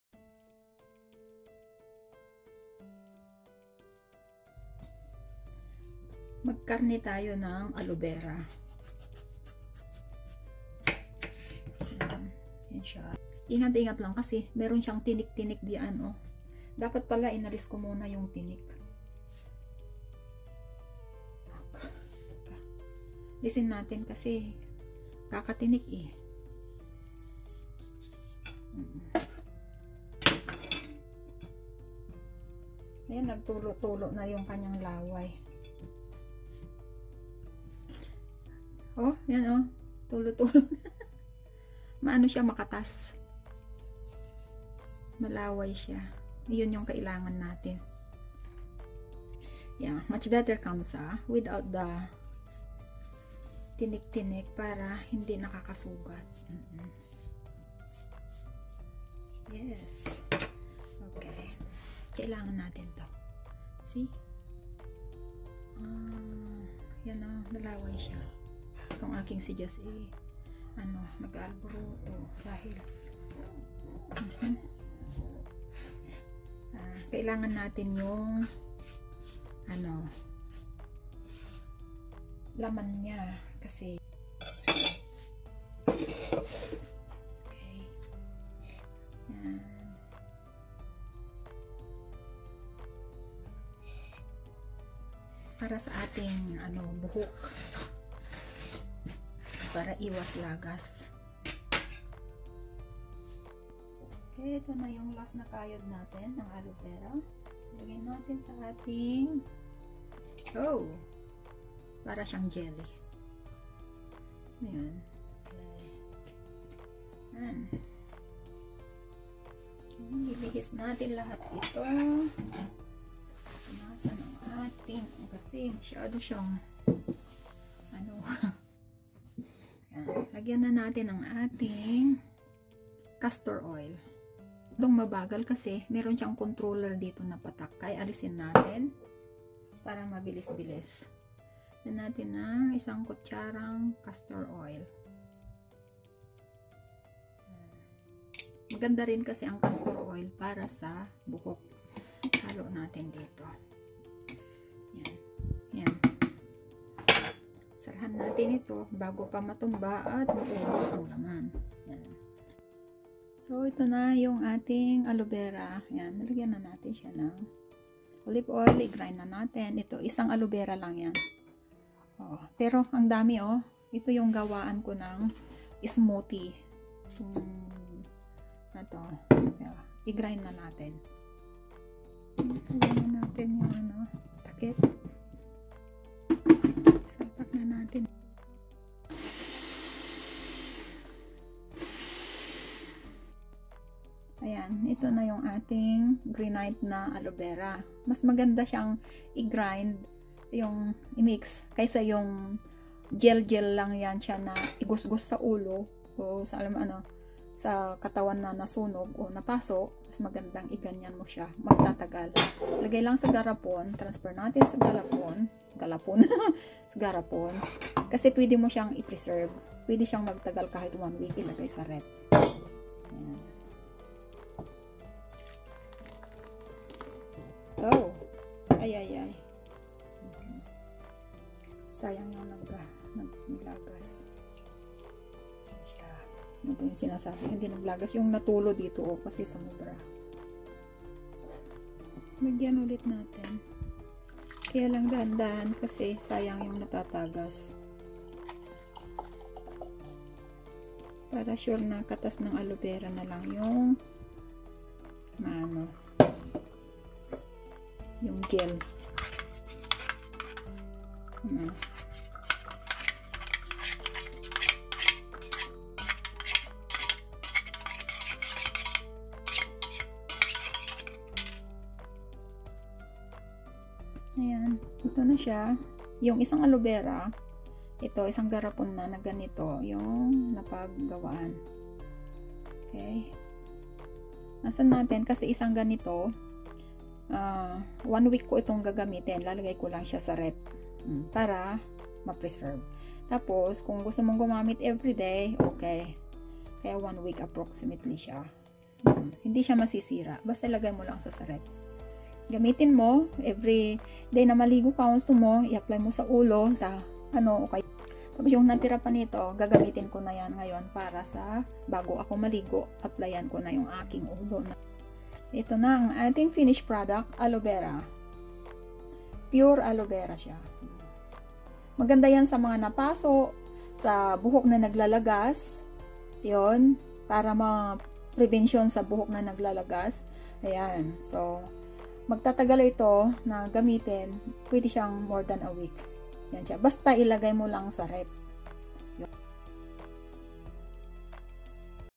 Fresh Aloe Vera Gel Benefits & How To Preserved Long Period of Time
Aloe vera gel, useful benefits of aloe vera gel. How to preserved fresh aloe Vera gel long time at home @titabhevschannel